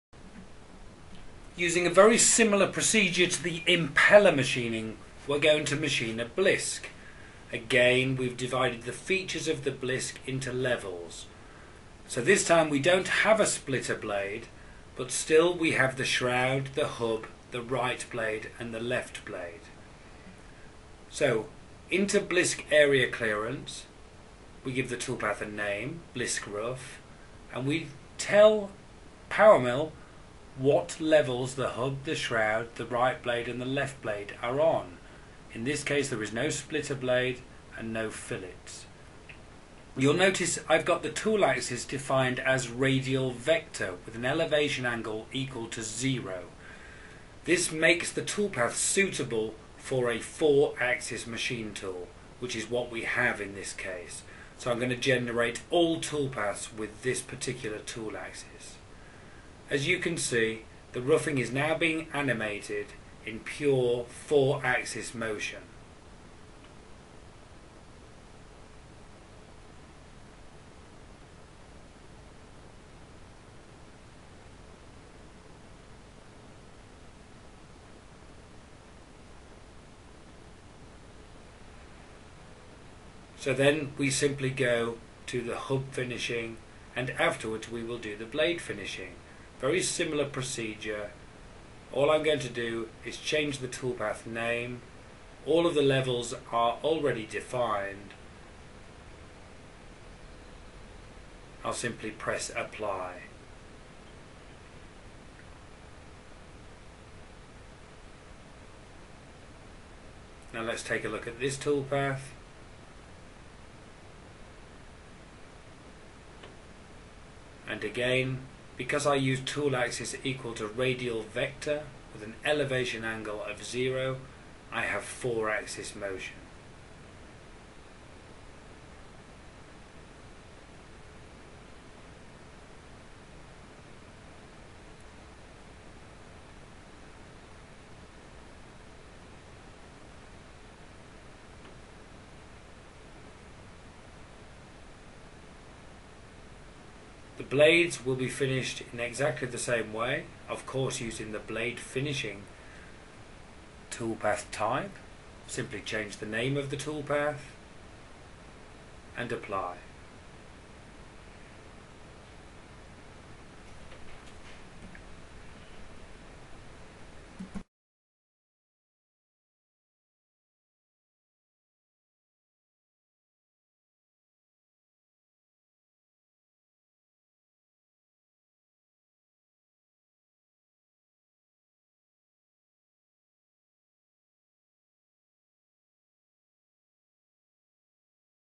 PowerMILL 5 Axis Overview Blisk Machining
New dedicated machining strategies for manufacturing blisk and impeller parts are now available in PowerMILL 8. The 3 machining strategies provide simultanious 5 axis Area Clearance as well as hub and blade finish machining of these complex parts.